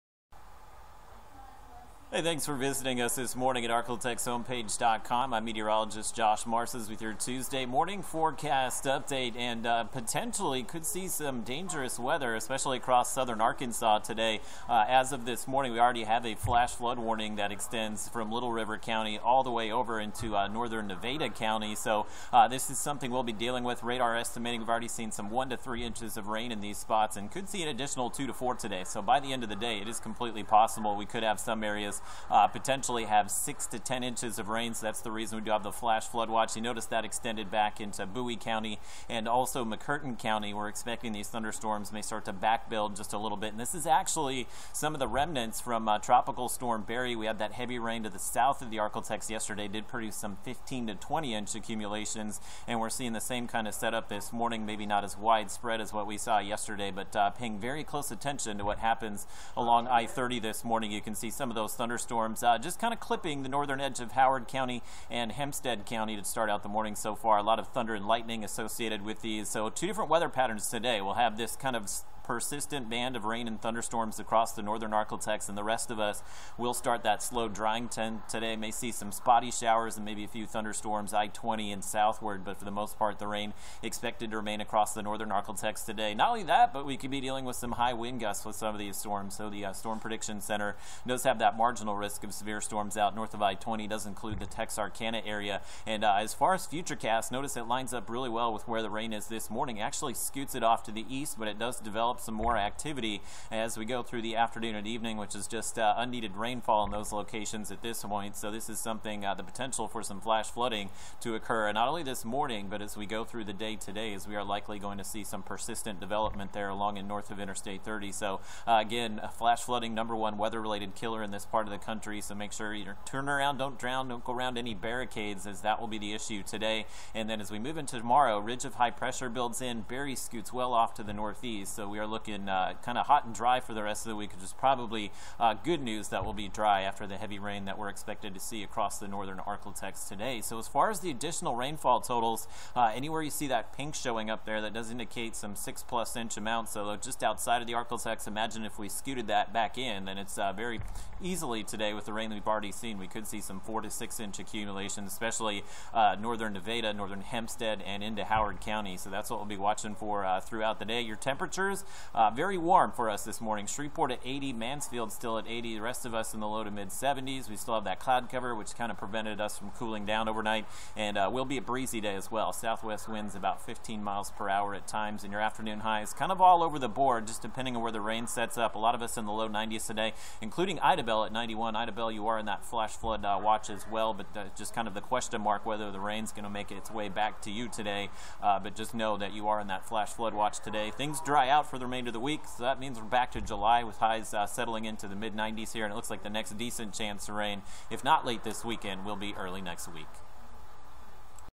Arklatex Outlook Tue 7/16 am
Weather webcast containing the latest seven day forecast for Shreveport, Texarkana, and all of the Arklatex.